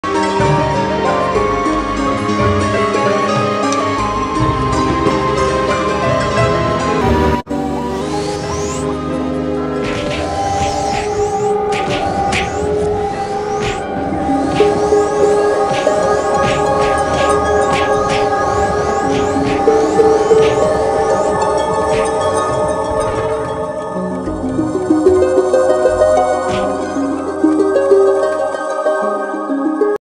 Devil Hand's Cast time bug!
Devil Hand was nerfed few weeks ago and made all its FCT reduction to just 50% and skill targeted. However, the 100% FCT seems to be activated in a random situation I don't understand. In the video, i suddenly gained 100% FCT after leveling up